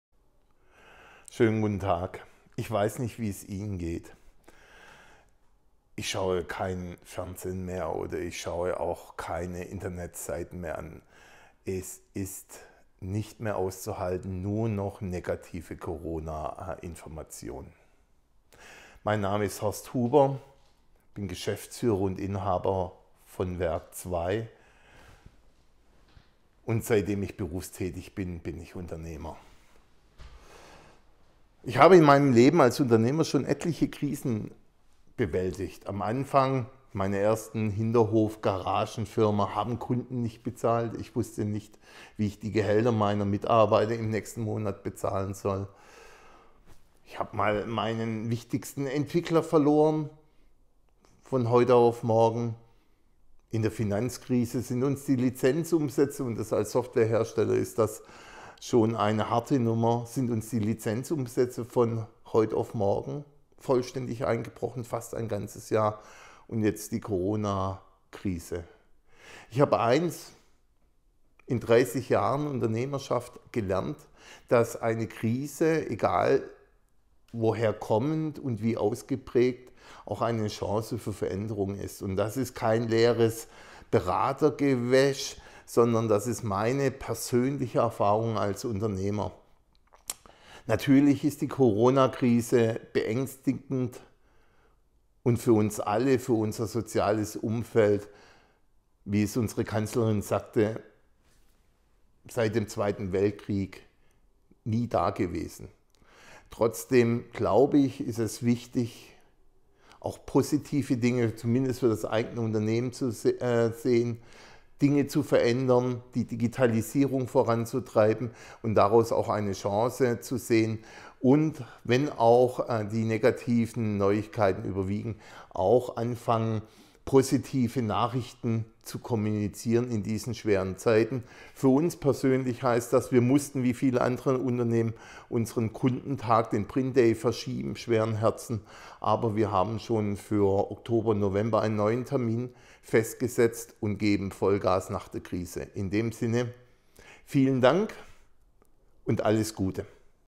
COVID19 Statement - Horst Huber, CEO & Founder
Unser CEO & Gründer Horst Huber mit ein paar kurzen Worten zu der momentanen Lage

Unser weltweites Team ist weiterhin für Sie erreichbar – schreiben Sie uns am besten eine Mail, damit wir aus dem Homeoffice antworten können.
Wir wünschen Ihnen und Ihren Familien viel Gesundheit.